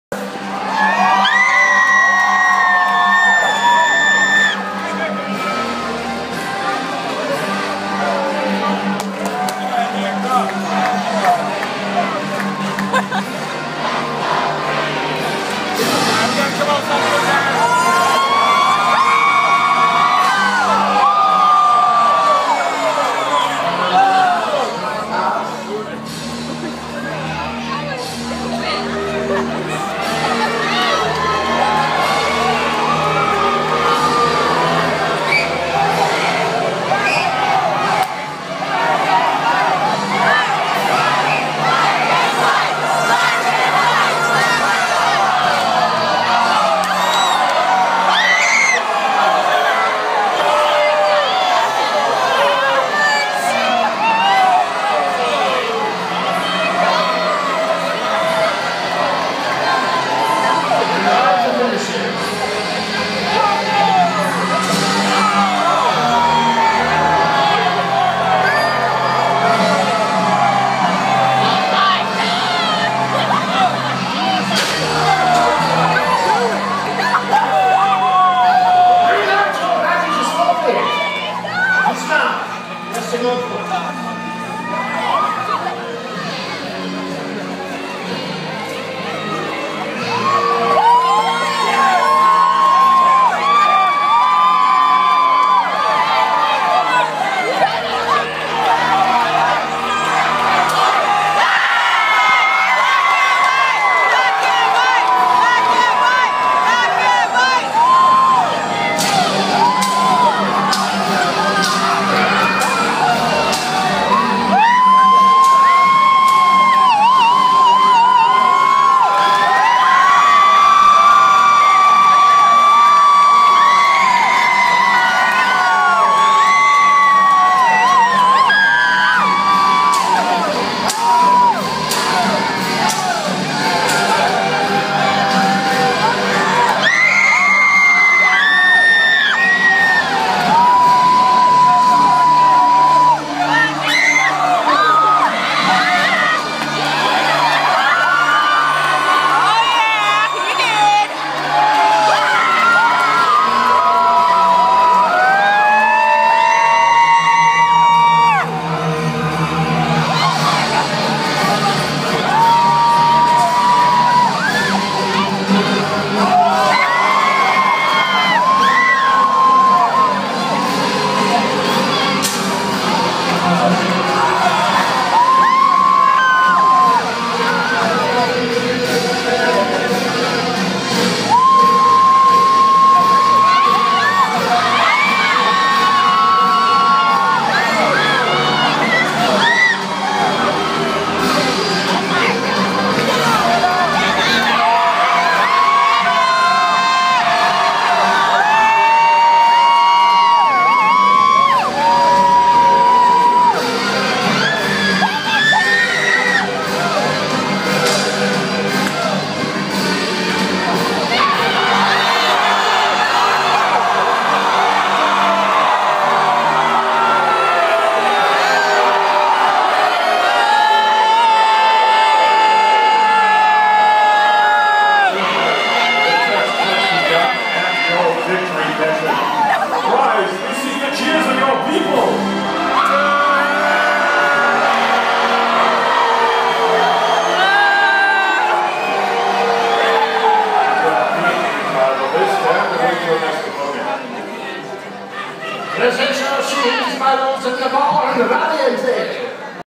White and Black Knight Jousting
Medieval Times in Buena Park, California Saturday May 24, 2014